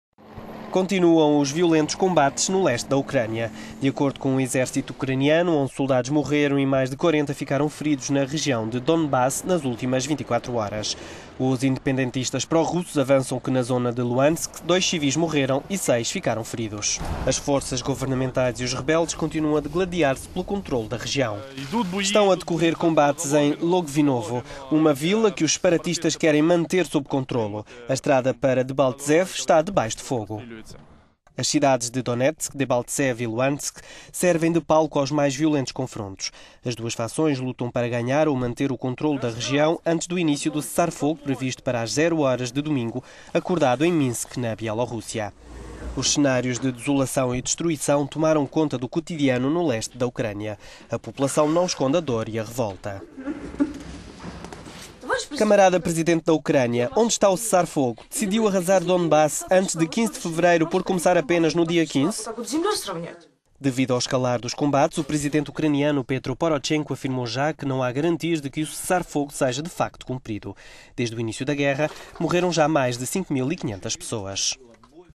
Ucrânia: Combates violentos antes do cessar-fogo
Continuam os violentos combates no leste da Ucrânia. Onze soldados morreram e mais de 40 ficaram feridos, na região de Donbass, nas últimas 24 horas, informou o exército ucraniano.

Os independentistas pró-russos avançam que, na zona de Luhansk, dois civis morreram e seis ficaram feridos.

As forças governamentais e os rebeldes pró-russos continuam a digladiar-se pelo controlo da região.

"Estão a decorrer combates em Logvinovo. Uma vila que os separatistas querem manter sob controlo. A estrad…